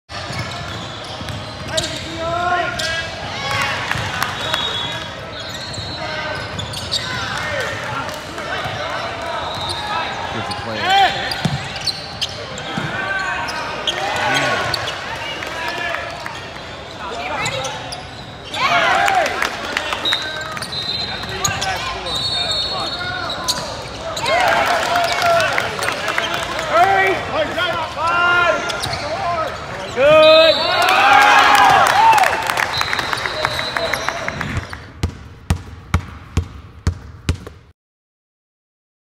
‪2021 Daimion Collins (Dallas Mustangs/Atlanta-TX) stood out in a big way at the UA Challenge
‪This kid already has a ton going for him and he is so young.‬

‪Watch his talent shine in these highlights (and enjoy one of the best dunks of the day).‬